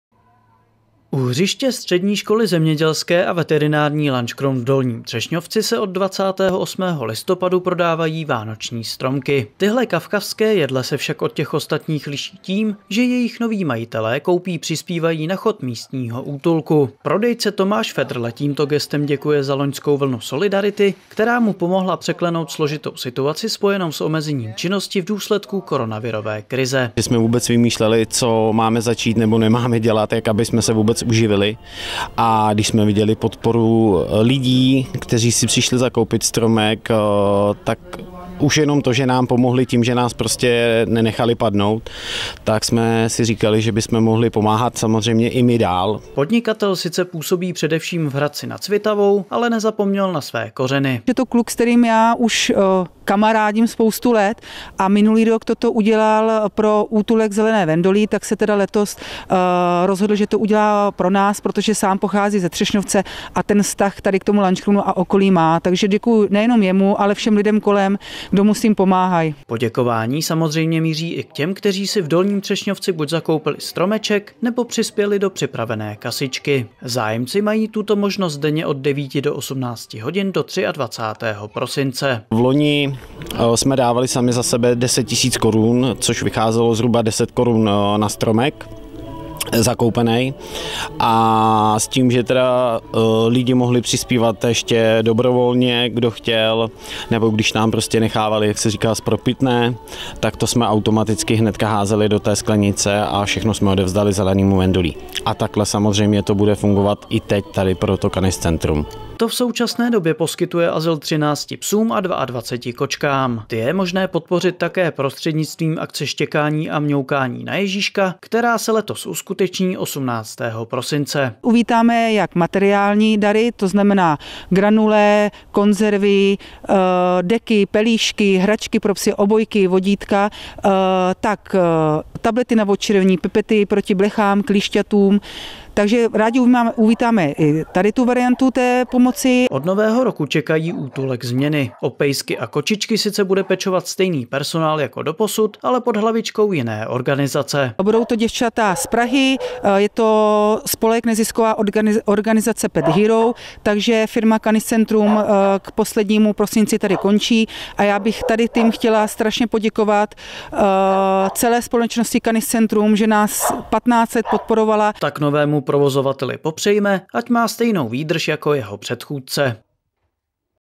Lanškroun: Pejsky a kočičky z útulku je možné podpořit i koupí vánočního stromku
Ještě nemáte vánoční stromeček a chtěli byste přispět na dobročinnost? Pokud ano, vydejte se do Dolního Třešňovce, kde se prodávají stromky, které pomáhají.